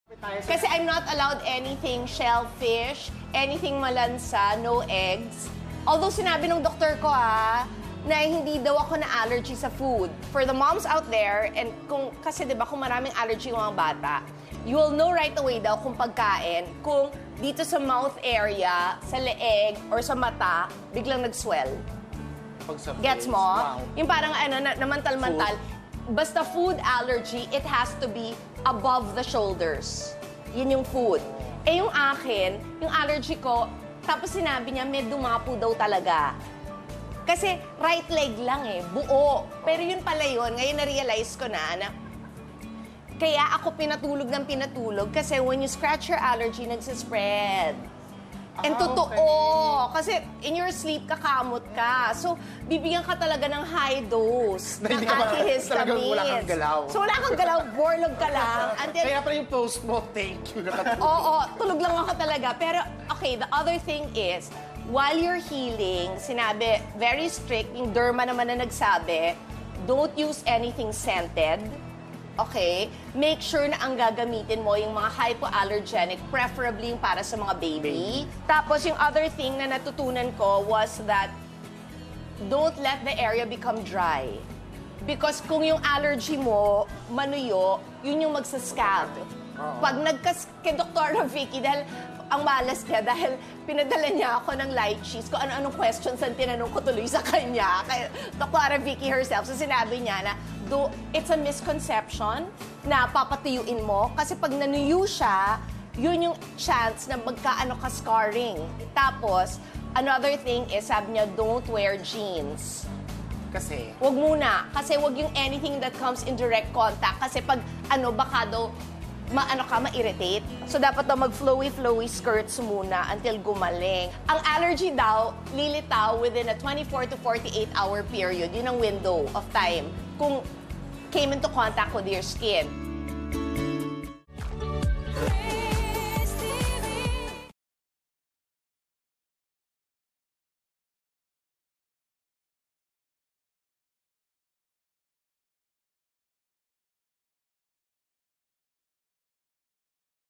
How to treat allergies according to Kris
Kris Aquino shares tips for treating allergies.